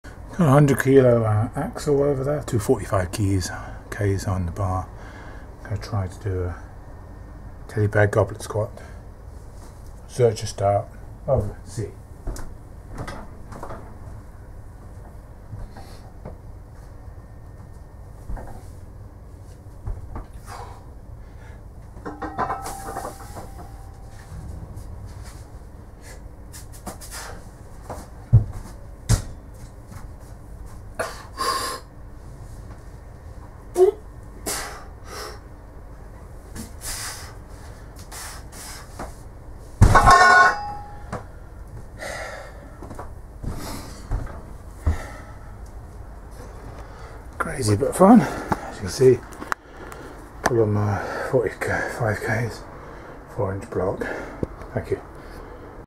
David Horne 100k Axle Teddy Bear Zercher Squat to 4in
David Horne 100k Axle Teddy Bear Zercher Squat to 4in block. Lift from the floor, and replace it.